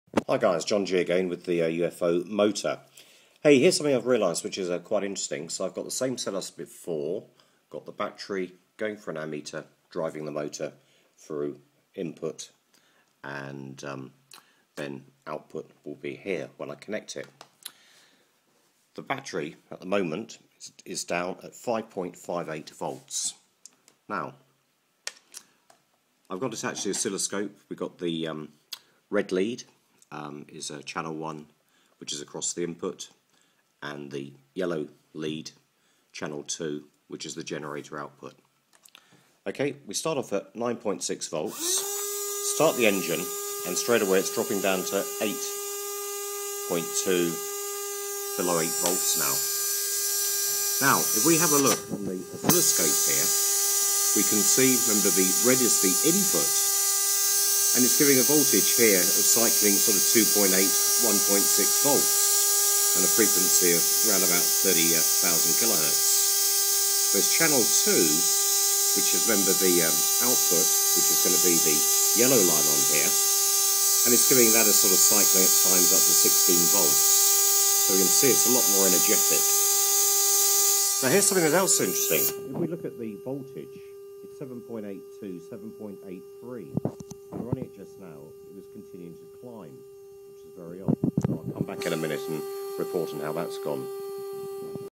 Initial Oscilloscope Test Part 1
Looking at the asymmetric motor with an oscilloscope